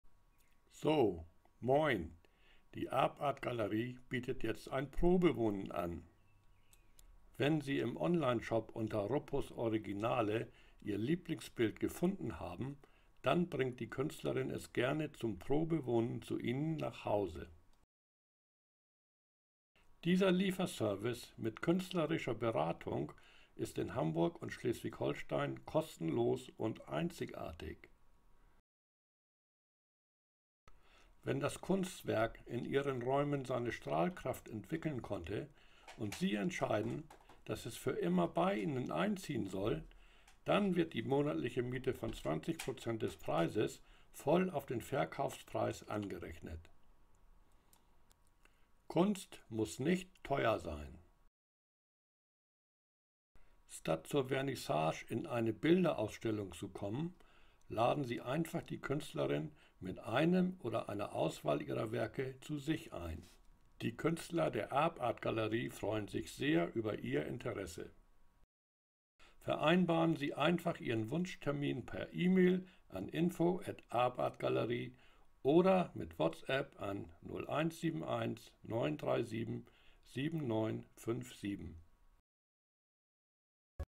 RUPPO(236)_Probewohnen 1
Wenn Sie im Online-Shop unter RUPPOs Originalen ihr Lieblingsbild gefunden haben, dann bringt die Künstlerin es gerne zum "Probewohnen" zu Ihnen nach Hause.

Dieser Liefer-Service mit künstlerischer Beratung ist in Hamburg und Schleswig-Holstein kostenlos. und einzigartig.

Wenn das Kunstwerk in Ihren Räumen seine Strahlkraft entwickeln konnte, und Sie entscheiden, dass es für immer bei Ihnen einziehen soll, dann wird die monatliche Miete von 20% voll auf den Verkaufspreis angerechnet.

Kunst muss nicht teuer sein.

Statt zur Vernissage in eine Bilder-Ausstellung zu kommen, laden Sie einfach die Künstlerin mit einem ihrer Werke zu sich ein.

oder mit WhatsApp an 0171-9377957.

Die Künstler der ARPart Gallery freuen sich über Ihr Interesse.